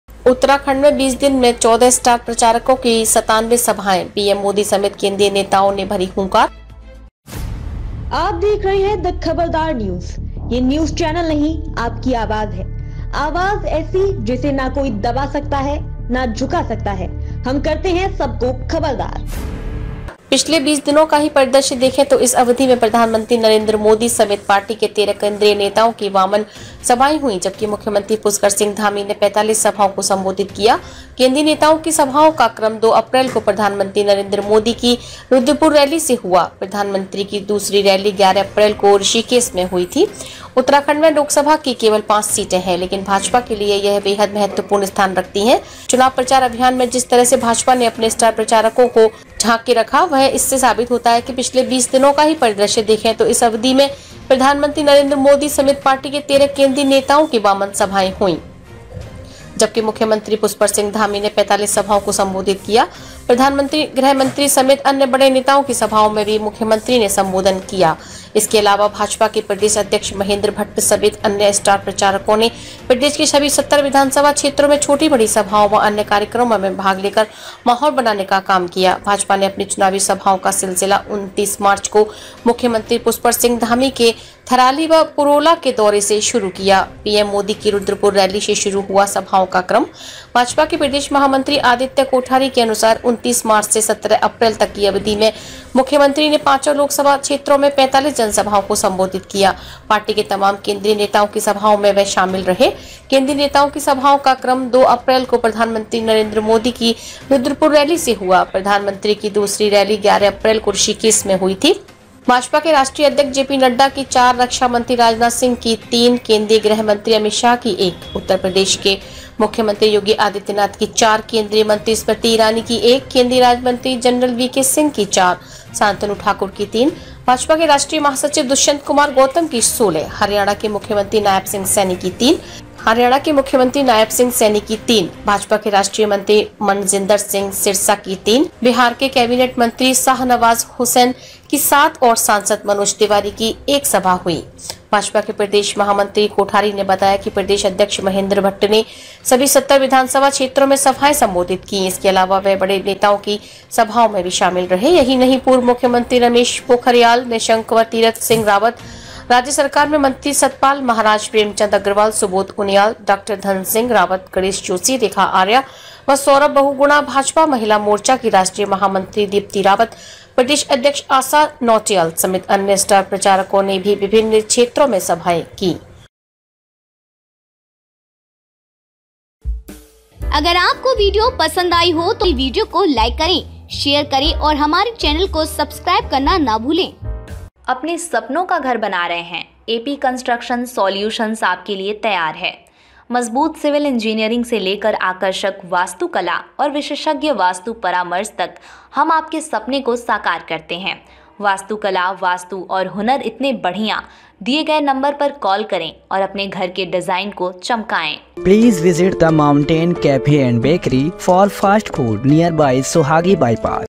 Lok Sabha Election 2024 : Uttarakhand में 20 दिन में 14 स्टार प्रचारकों की 97 सभाएं,
भाजपा के प्रदेश महामंत्री आदित्य कोठारी के अनुसार 29 मार्च से 17 अप्रैल तक की अवधि में मुख्यमंत्री ने पांचों लोकसभा क्षेत्रों में 45 जनसभाओं को संबोधित किया। पार्टी के तमाम केंद्रीय नेताओं की सभाओं में वह शामिल रहे।  केंद्रीय नेताओं की सभाओं का क्रम दो अप्रैल को प्रधानमंत्री नरेन्द्र मोदी की रुद्रपुर रैली से हुआ। प्रधानमंत्री की दूसरी रैली 11 अप्रैल को ऋषिकेश में हुई थी।